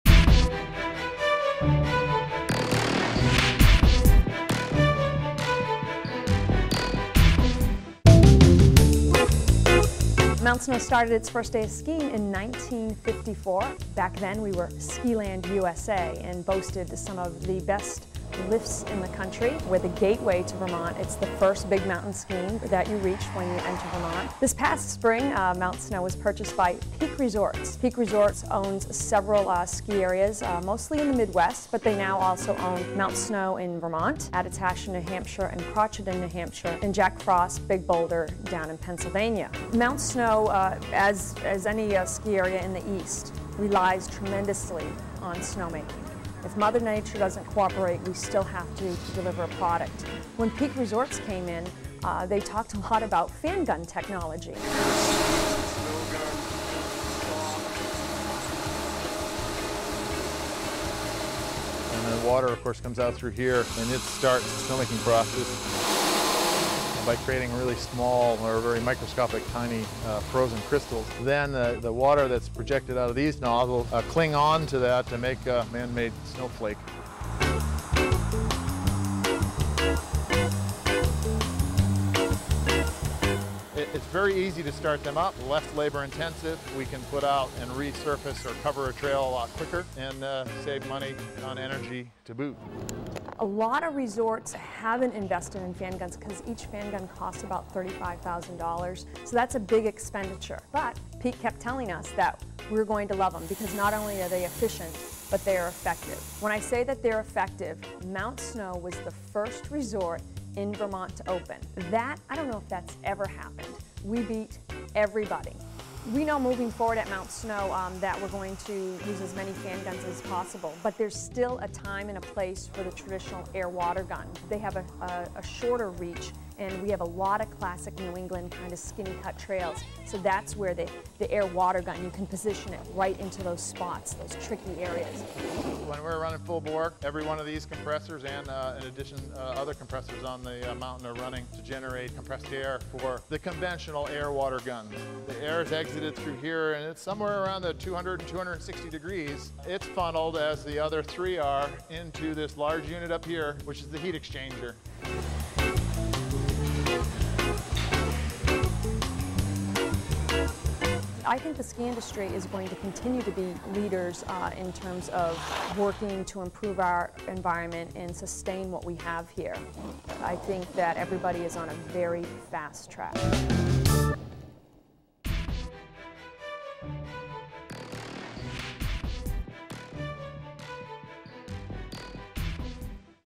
CNNMoney.com Green Skiing at Mount Snow
CNNMoney.com Green Skiing at Mount Snow. Fan guns, Peak Resorts and great skiing...